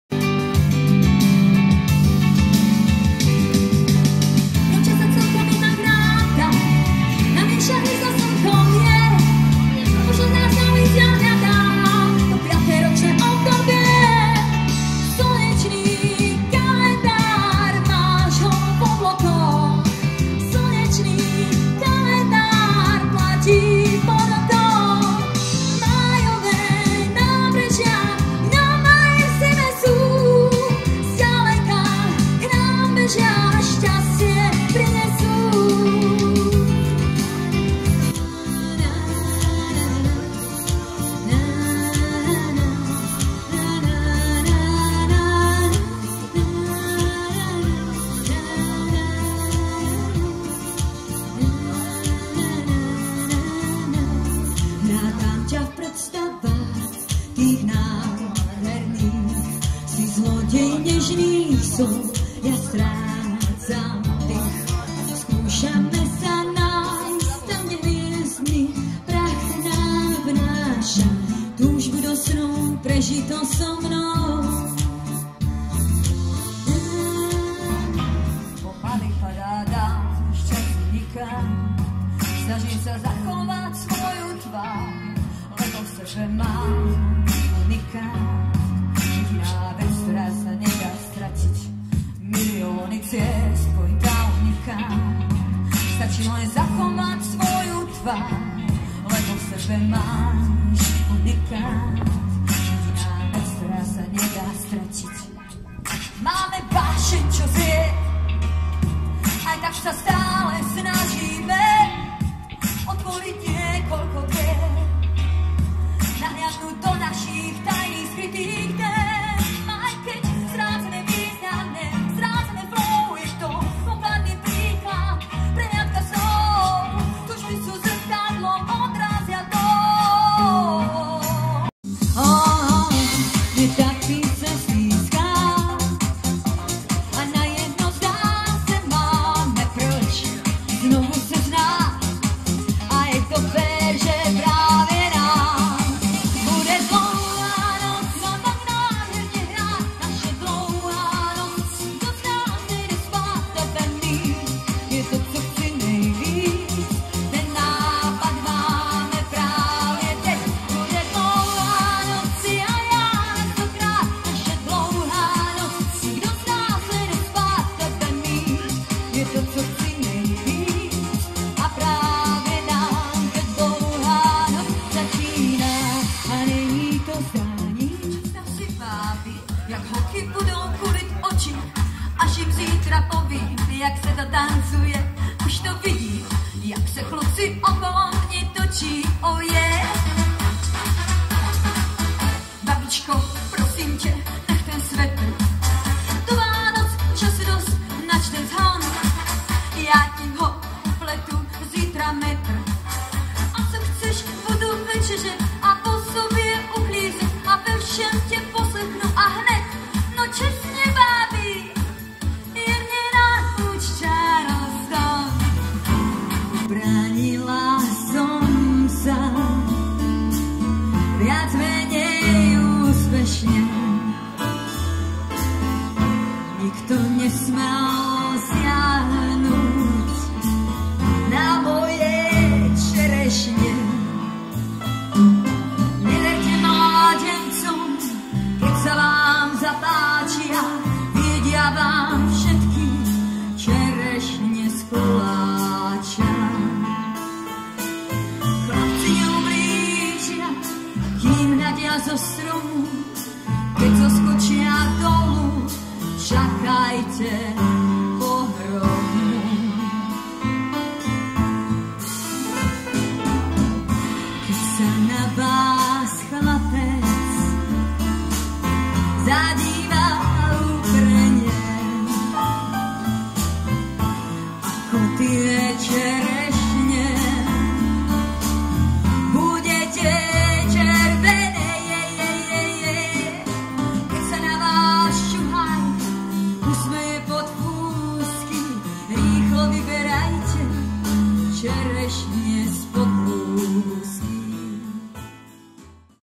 Futbalový turnaj TJ Visolaje
Ukážka vystúpenia z Futbalového turnaja TJ Visolaje 70.výročie. Futbalové polčasy vyplnila speváčka Janka Štefancová